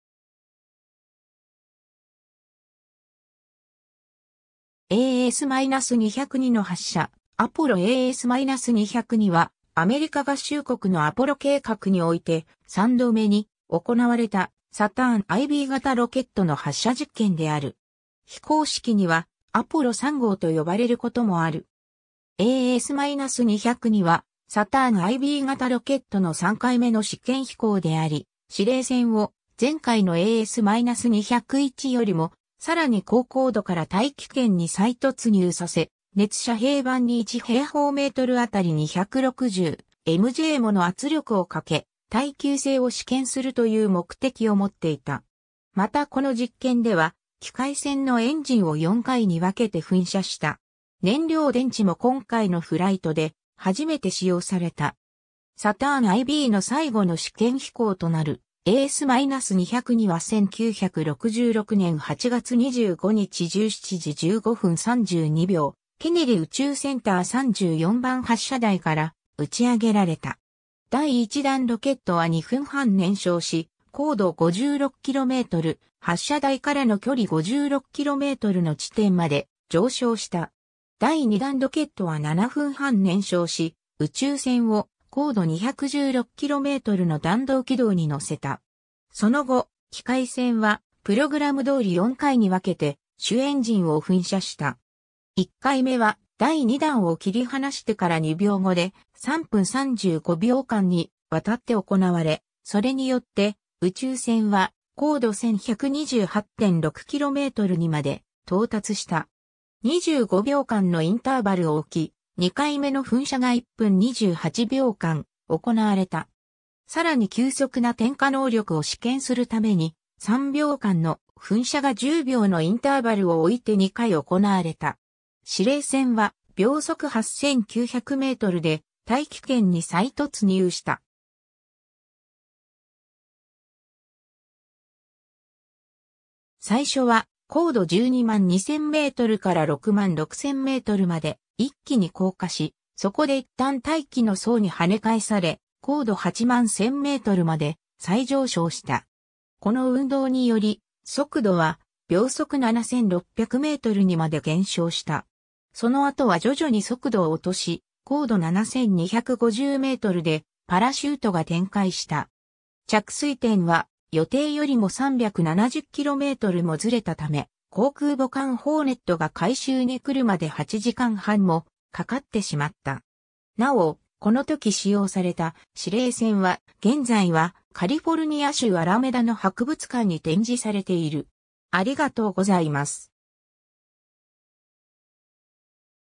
アポロAS-202
AS-202の発射 アポロAS-202はアメリカ合衆国のアポロ計画において、三度目に行われたサターンIB 型ロケットの発射実験である。
非公式にはアポロ3号と呼ばれることもある。
 AS-202はサターンIB 型ロケットの3回目の試験飛行であり、司令船を前回のAS-201よりもさらに高高度から大気圏に再突入させ、熱遮蔽板に1平方メートルあたり260MJもの圧力をかけ、耐久性を試験するという目的を持っていた。
またこの実験では、機械船のエンジンを4回に分けて噴射した。
燃料電池も今回のフライトで初めて使用された。
 サターンIB の最後の試験飛行となるAS-202は1966年8月25日17:15:32（UTC）、ケネディ宇宙センター34番発射台から打ち上げられた。
第一段ロケットは2分半燃焼し、高度56km、発射台からの距離56kmの地点まで上昇した。
第二段ロケットは7分半燃焼し、宇宙船を高度216kmの弾道軌道に乗せた。
 その後、機械船はプログラムどおり4回に分けて主エンジンを噴射した。
1回目は第二段を切り離してから2秒後で、3分35秒間にわたって行われ、それによって宇宙船は高度1,128.6kmにまで到達した。
25秒間のインターバルを置き、2回目の噴射が1分28秒間行われた。
さらに急速な点火能力を試験するために、3秒間の噴射が10秒のインターバルを置いて2回行われた。
 司令船は秒速8,900mで大気圏に再突入した。
最初は高度122,000mから66,000mまで一気に降下し、そこでいったん大気の層にはね返され、高度81,000mまで再上昇した。
この運動により、速度は秒速7,600mにまで減少した。
その後は徐々に速度を落とし、高度7,250mでパラシュートが展開した。
着水点は予定よりも370kmもずれたため、航空母艦ホーネット（USS Hornet）が回収に来るまで8時間半もかかってしまった。
 なお、この時使用された司令船は、現在はカリフォルニア州アラメダの博物館に展示されている。